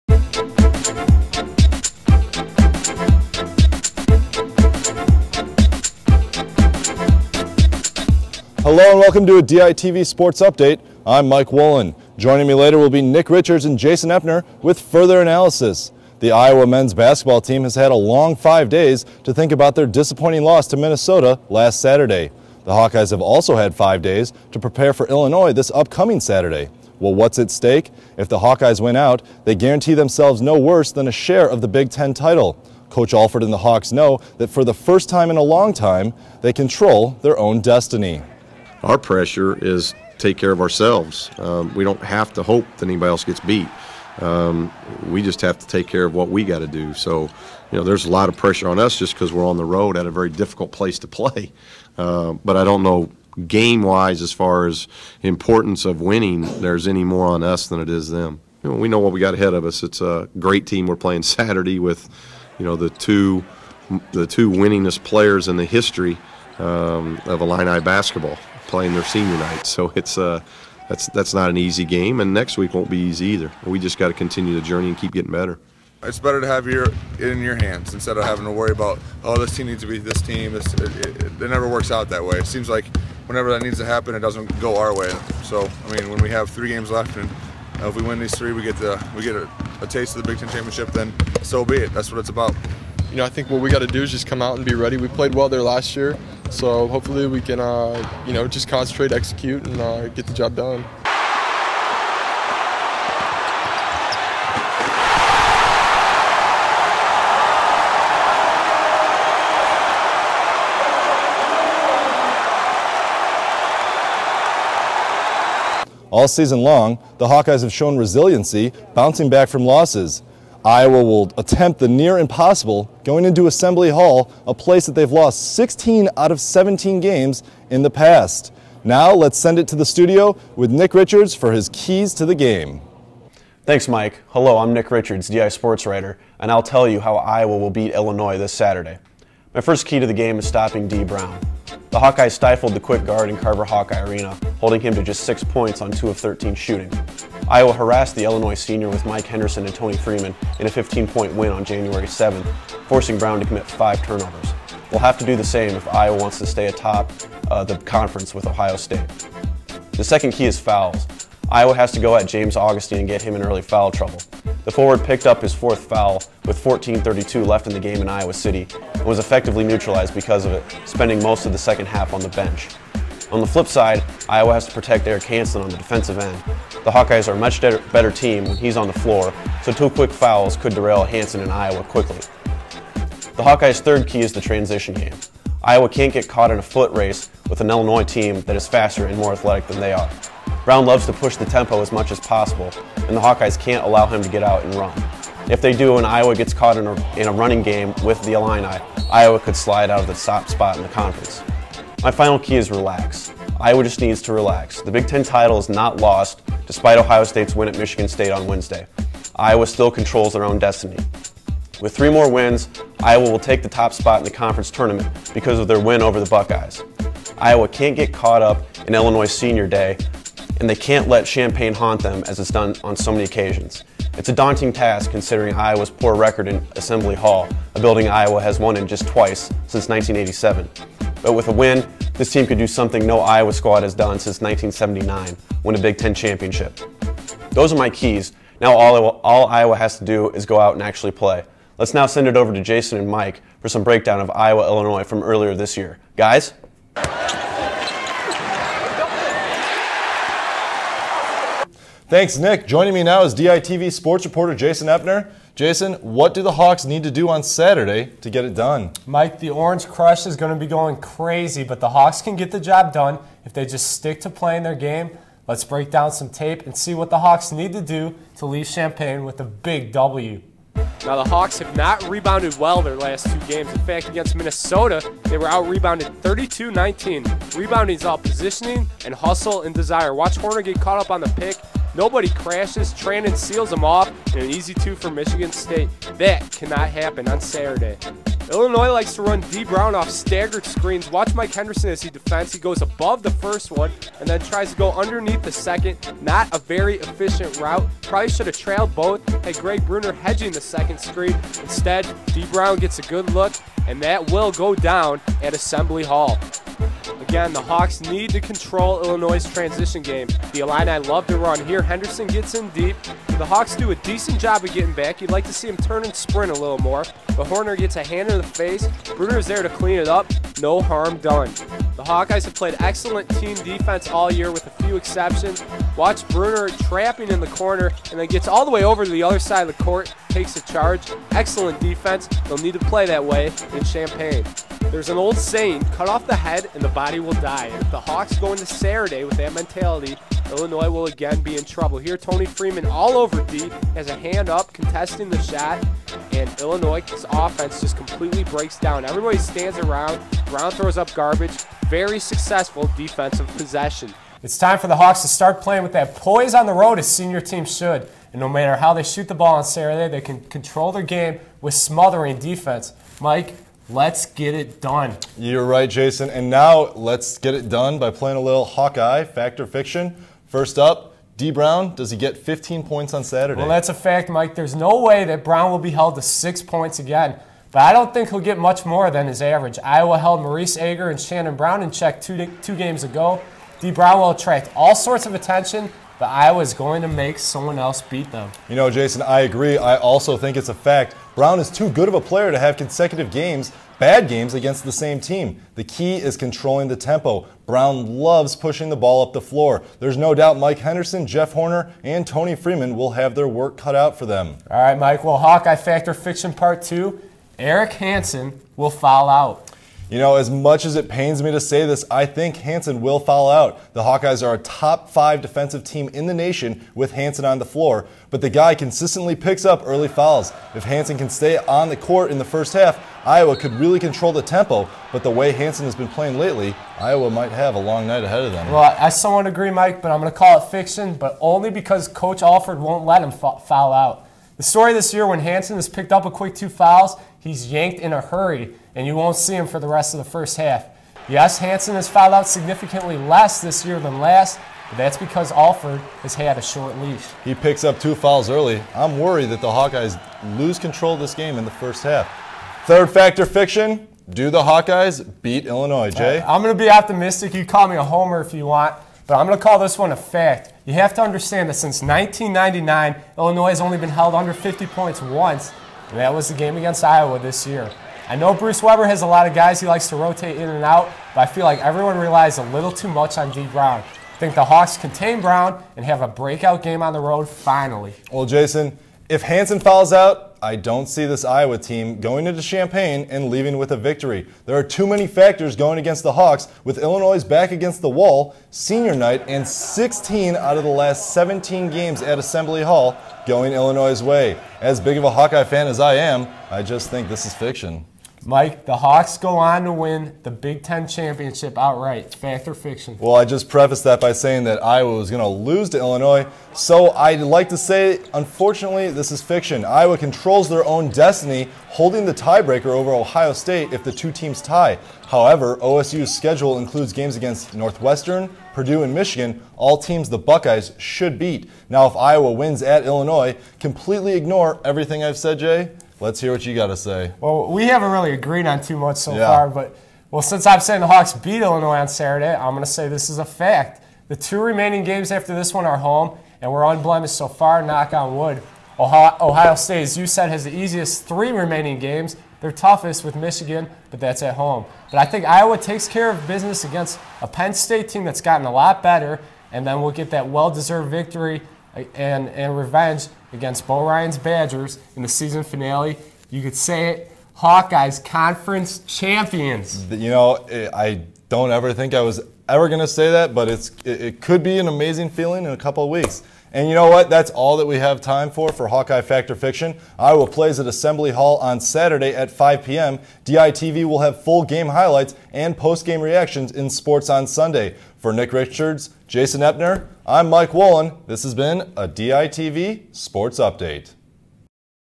Men's Basketball Preview: Iowa vs. Illinois
We take a look at this weekend's upcoming game against Illinois.